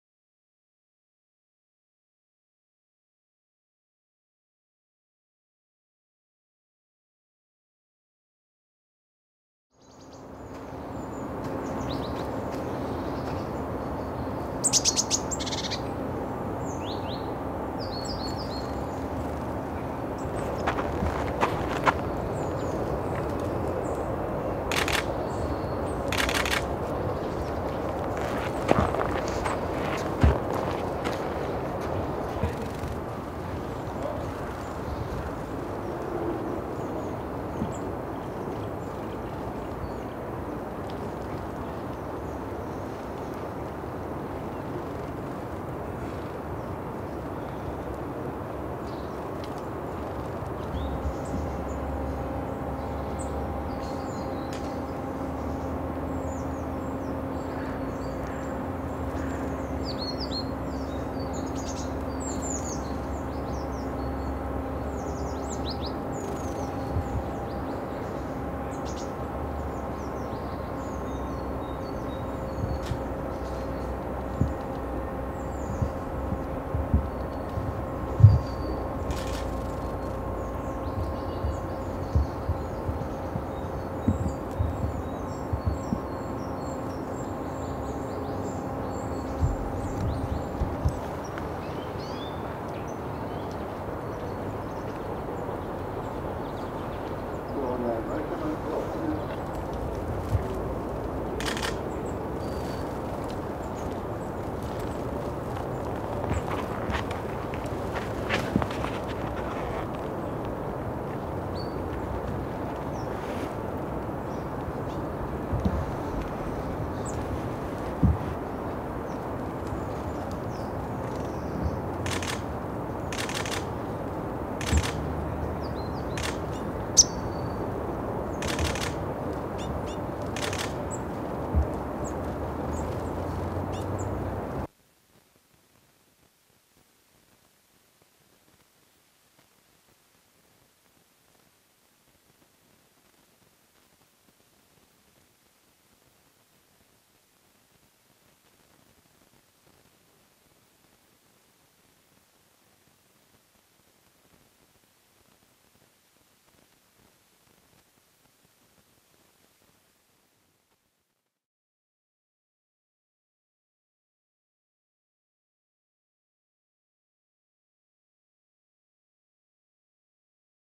DVÄRGSPARV Little Bunting (Emberiza pallasi) Klipp - 1577
Dvärgsparv. December i Uppland. (A-3). Little Bunting in Sweden,  AROS FILM Fotograf: Gunnar Fernqvist. Västerås. Sweden.